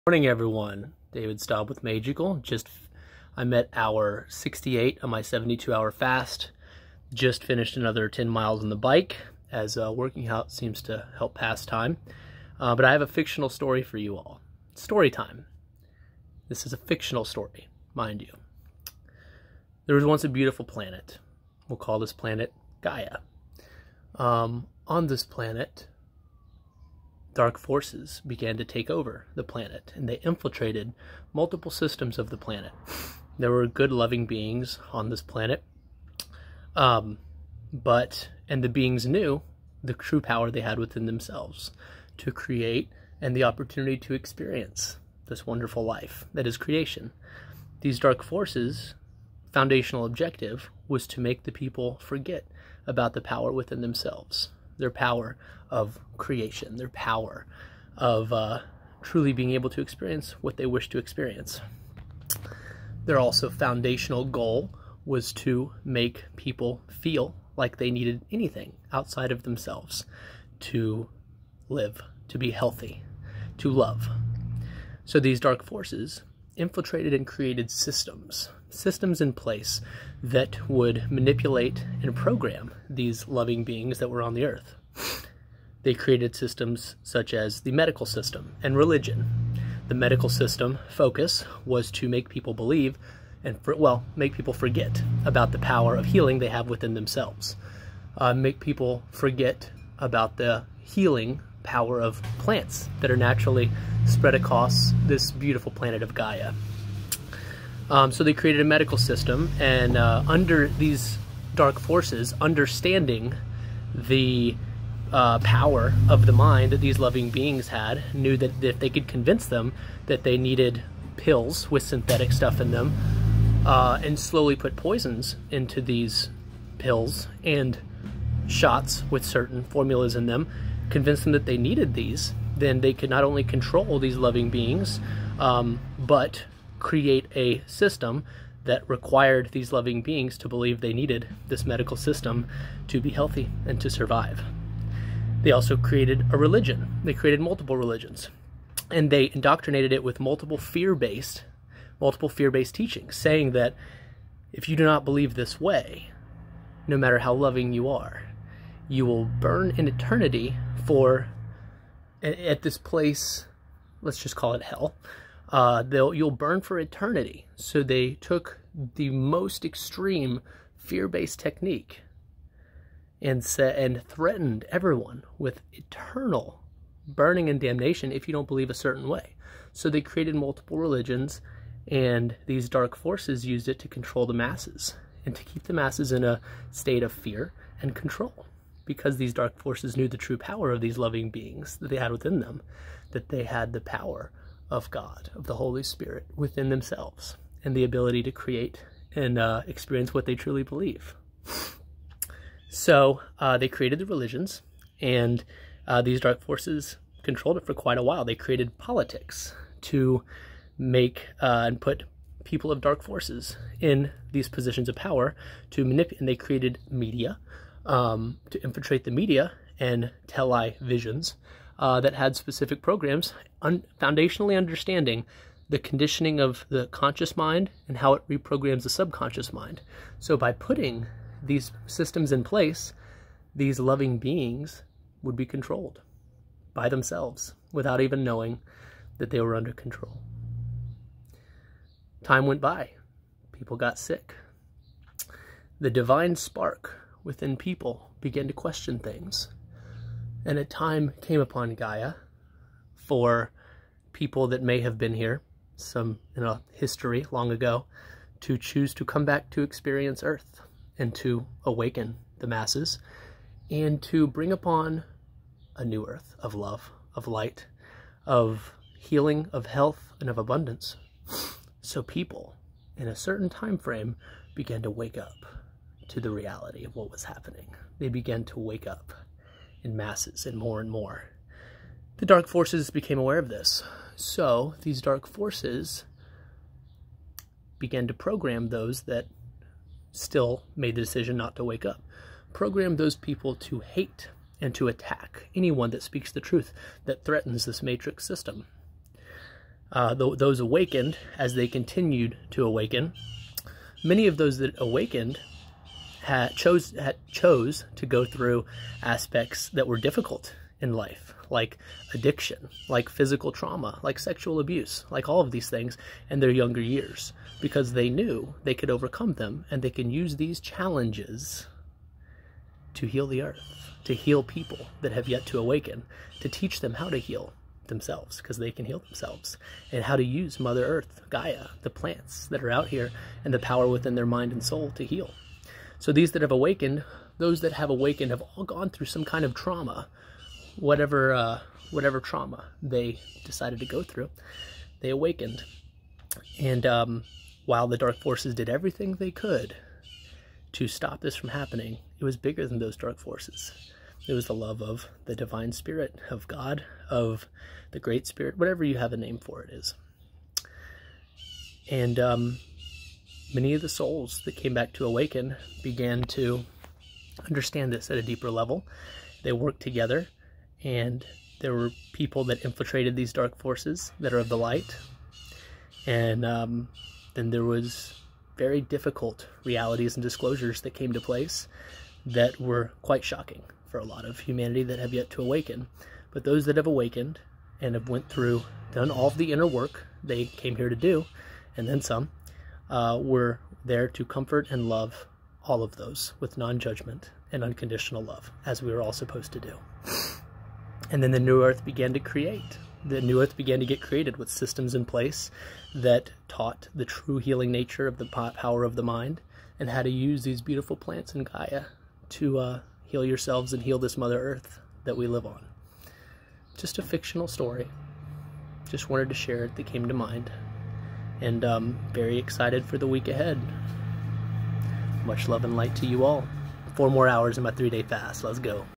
A "Fictional" story of a planet called Gaia, war of dark and light
A fictional story of a planet called "Gaia" and a war between dark and light.

much love to you all in the coming days of revealing